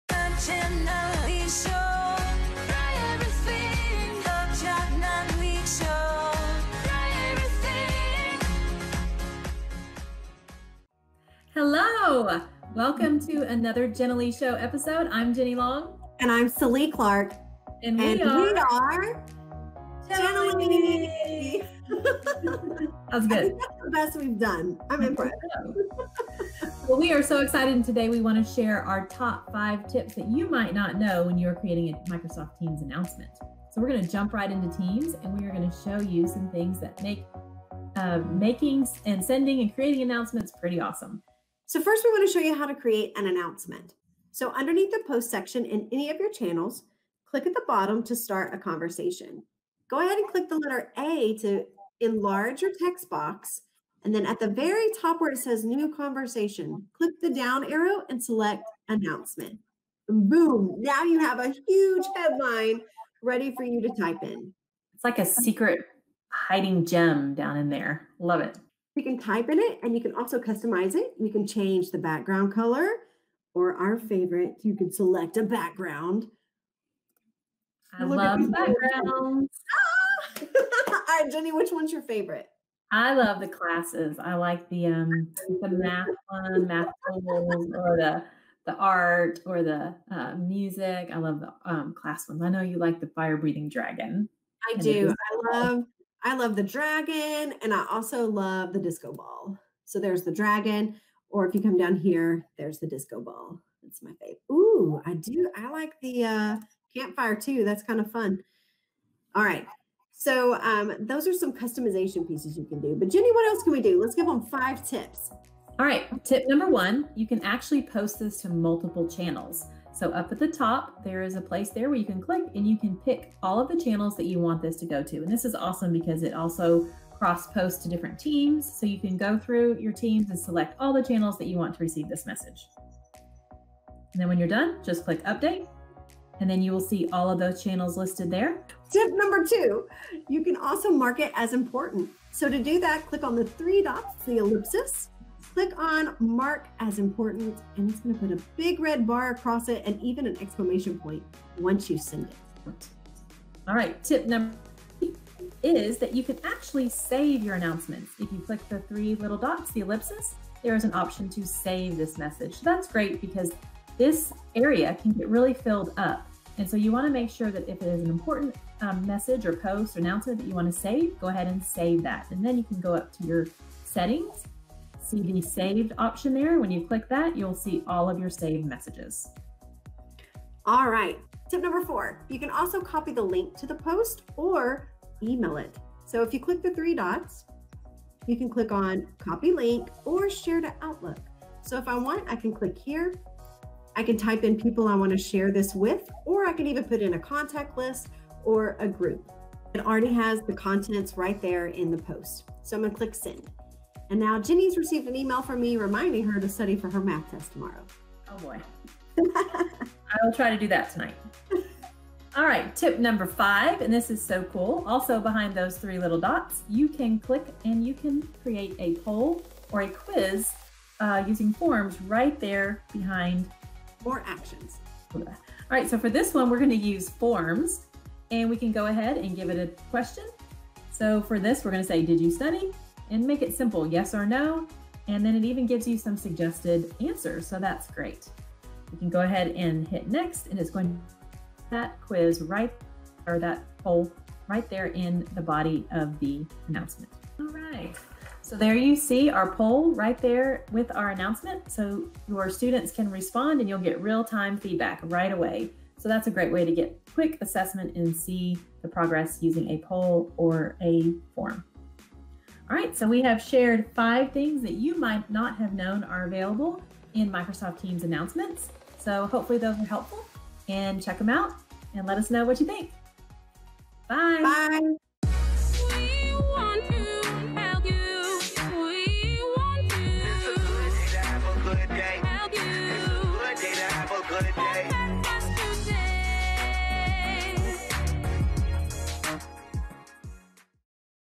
5 Teams Announcement Features!
Did you know these 5 awesome features were built into Teams Announcements? Check it out!